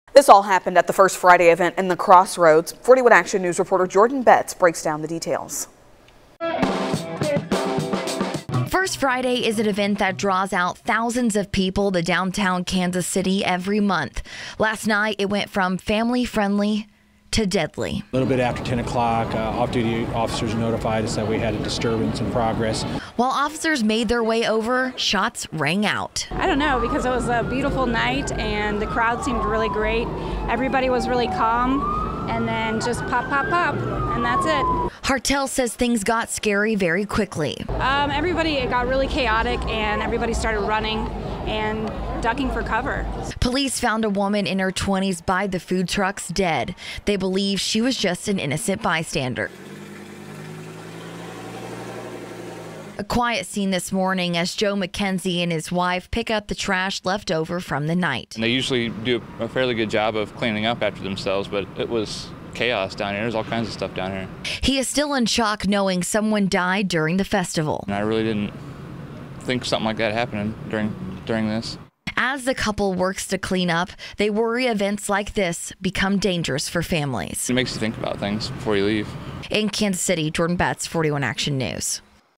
Family friendly event turns deadly
One person was killed when shots broke out at First Fridays, which is attended by thousands each month.